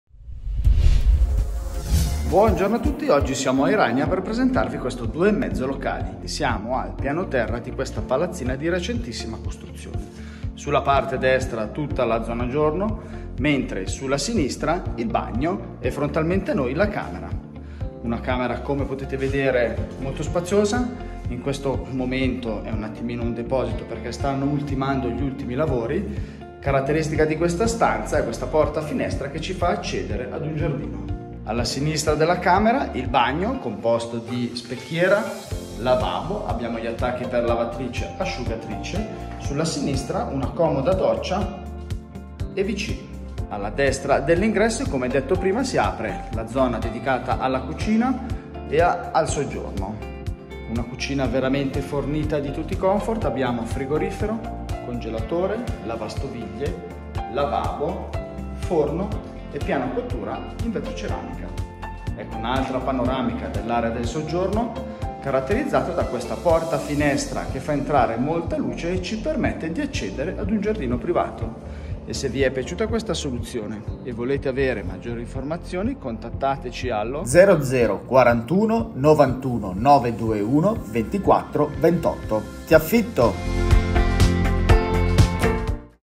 NUOVISSIMO 2.5 CON GIARDINO AD IRAGNA
Siamo ad Iragna,
avvolti da sentieri e montagne, dimora perfetta per gli spiriti liberi che vogliono scappare dal caos cittadino pur non allontanandosi dalle comodità.

La residenza di NUOVISSIMA COSTRUZIONE sorge in posizione strategica a meno di 5km da Biasca e circa 15km da Bellinzona.

La zona, pur estremamente intima e tranquilla, non manca di Servizi essenziali, mezzi pubblici, negozi.

Questo ampio 2.5 locali CON GIARDINO è perfetto per coloro che cercano una casa da abitare ma soprattutto da vivere.

La moderna concenzione di costruzione permette di vivere in ambienti ad IMPATTO ECOLOGICO RIDOTTO, con consumi molto bassi rispetto ai tradizionali costi di gestione.

Il riscadalmento a termopompa scalda gli appartamenti tramite serpentine a pavimento, ed è a sua volta alimentato da pannelli fotovoltaici che permettono di abbattere considerevolmente i costi.

L’ingresso offre un armadio a muro dove liberarsi della giornata.

LA ZONA GIORNO -in concezione moderna- è open-space, le vetrate lasciano entrare la luce del sole creando un ambiente armonioso.

La cucina, a vista è modernissima e completa di ogni elettrodomestico.

Dal soggiorno la grande vetrata scorrevole accompagna verso la GRANDE TERRAZZA seguita da un grazioso giardino condominiale.

IL BAGNO serve questa zona ed è completo di lavabo con specchiera, wc e doccia.
Qui sono presenti gli attacchi per la colonna lavasciuga.

Infine è presente una CAMERA MATRIMONIALE.

Questo ampio 2.5 locali viene affittato a 1’050 franchi, più le spese accessorie di 100 franchi mensili.

È disponibile un posto auto esterno al costo di 50 franchi mensili.

Gli ANIMALI di piccola taglia sono ammessi.

È disponibile da SUBITO.